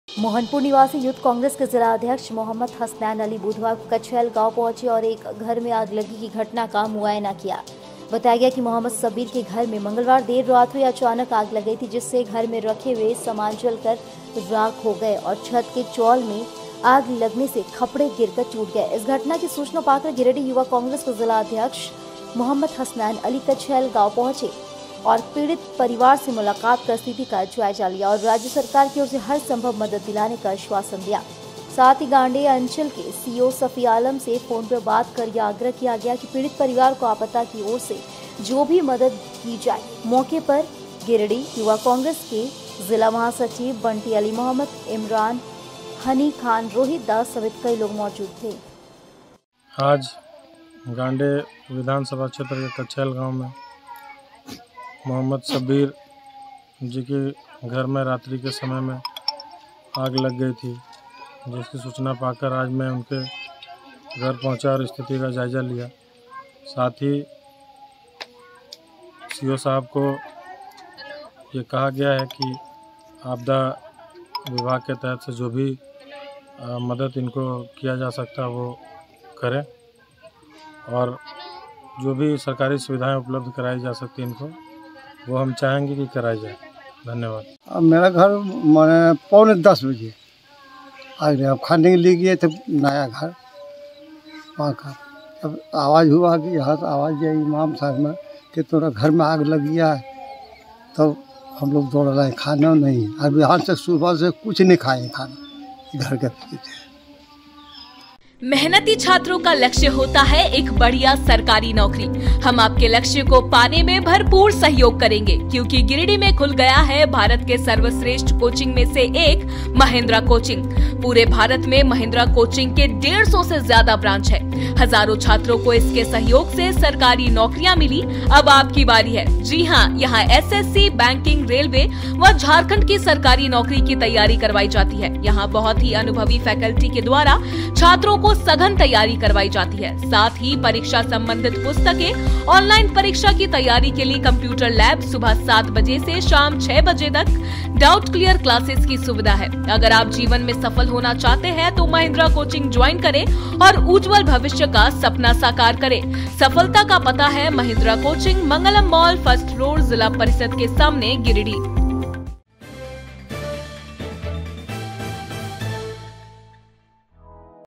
Giridih मो हसनैन अली कछैल गांव पहुंचकर एक घर में आगलगी की घटना का मुआयना किया
मोहनपुर निवासी यूथ कांग्रेस के जिलाध्यक्ष मो हसनैन अली बुधवार को कछैल गांव पहुंचकर एक एक में आगलगी की घटना का मुआयना किया।